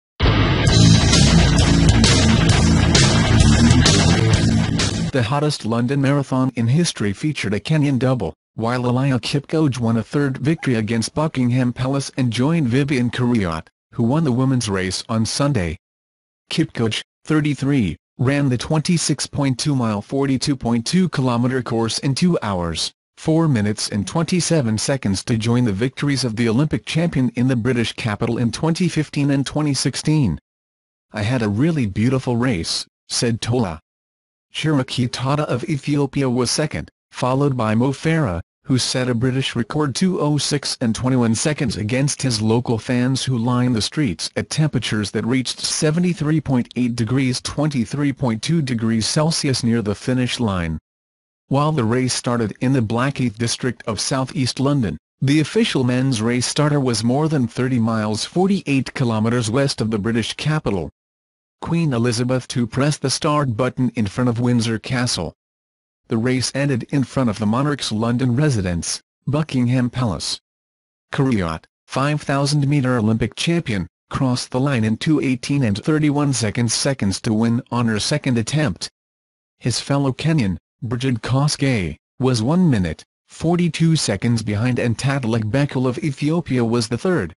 The hottest marathon in London had a Kenyan double Eliud Kipchoge achieved third victory.
The hottest London marathon in history featured a Kenyan double, while Eliud Kipchoge won a third victory against Buckingham Palace and joined Vivian Cheruiyot, who won the women's race on Sunday. Kipchoge, 33, ran the 26.2-mile 42.2-kilometer course in 2 hours, 4 minutes and 27 seconds to join the victories of the Olympic champion in the British capital in 2015 and 2016. I had a really beautiful race, "said Tola. Shura Kitata of Ethiopia was second, followed by Mo Farah, who set a British record 2:06:21 against his local fans who lined the streets at temperatures that reached 73.8 degrees 23.2 degrees Celsius near the finish line.

While the race started in the Blackheath district of south-east London, the official men's race starter was more than 30 miles 48 kilometers west of the British capital. Queen Elizabeth II pressed the start button in front of Windsor Castle. The race ended in front of the monarch's London residence, Buckingham Palace. Cheruiyot, 5,000-meter Olympic champion, crossed the line in 2:18:31 seconds to win on her second attempt. His fellow Kenyan, Brigid Kosgei, was 1 minute, 42 seconds behind and Tadelech Bekele of Ethiopia was the third.